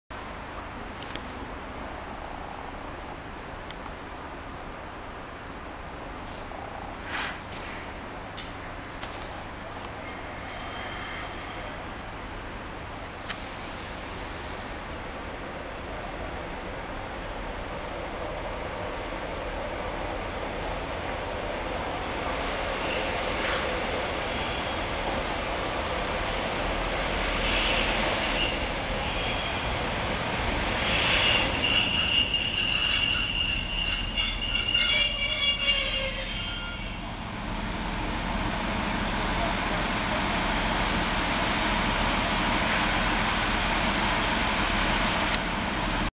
Final Class 144's Northern Class 144002 and 144005 passing through Rotherham Central - Part 1
Hello Everyone

This is a Video Showing the Final two Class 144s to be Moved from Keighley and Worth Heritage Railway. this is Located Near Leeds in West Yorkshire.

This Part shows the Class 144002 (at Front) and Class 144005 (at Rear) pulling into Rotherham Central to drop some Northern staff off and to Change Drivers.